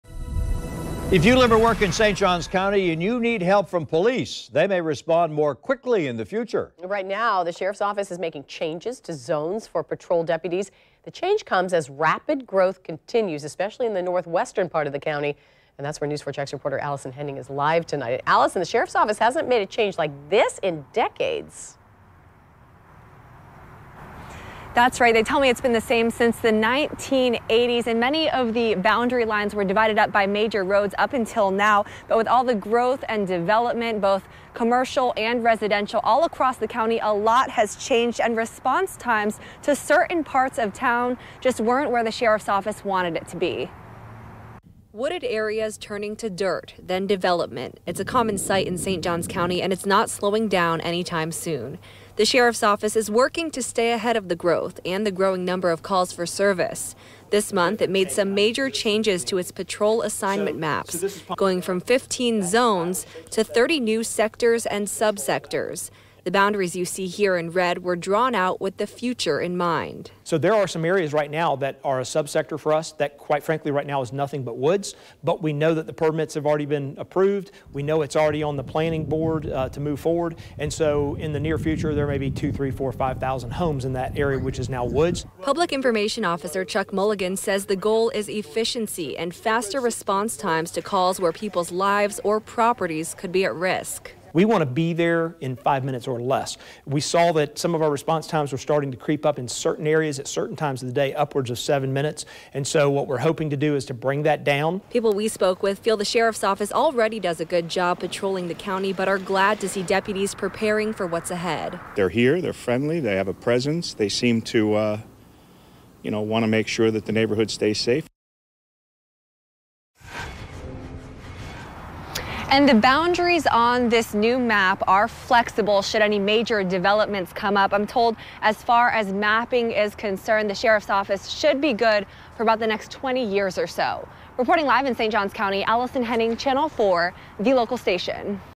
Sheriff changing patrol zones in St. Johns
The St. Johns County sheriff's office is making changes to zones for patrol deputies. The change comes as rapid growth continues, especially in the northwestern part of the county.